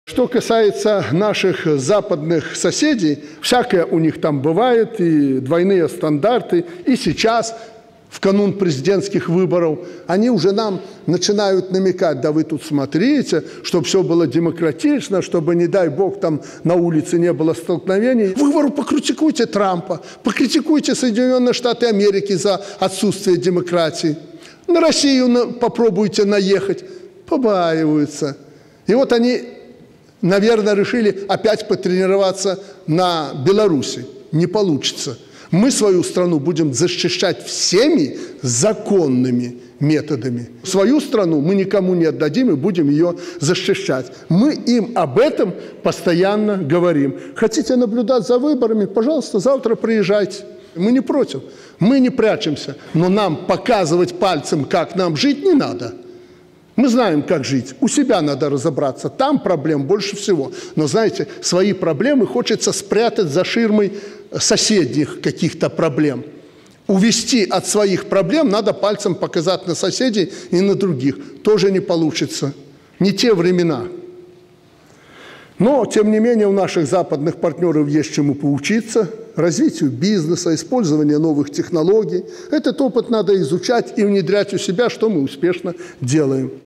Лукашенко: свою страну мы никому не отдадим и будем ее защищать
Свою страну никому не отдадим и будем ее защищать. Об этом Президент Беларуси Александр Лукашенко заявил на встрече с активом Витебской области.